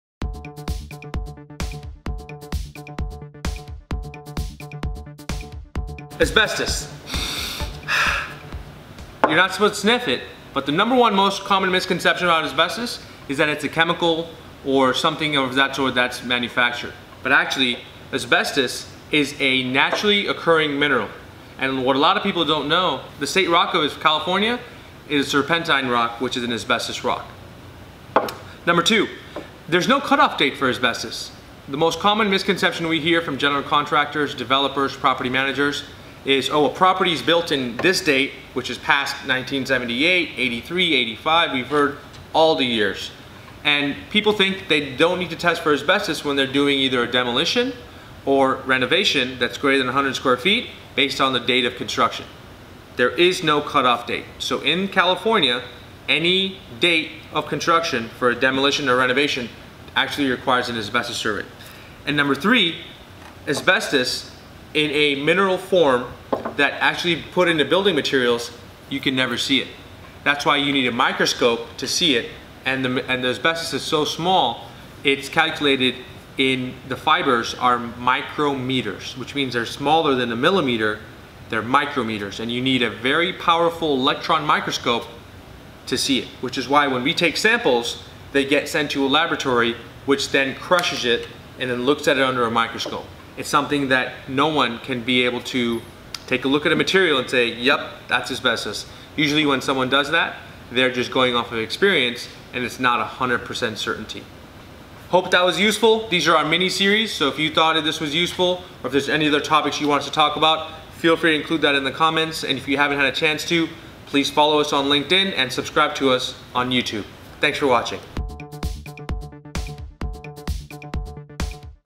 3 Misconceptions About Asbestos
There are many misconceptions about Asbestos and how it impacts buildings, air quality and overall development.  We go over the most common misconceptions and in the process - I sniff some naturally occurring asbestos.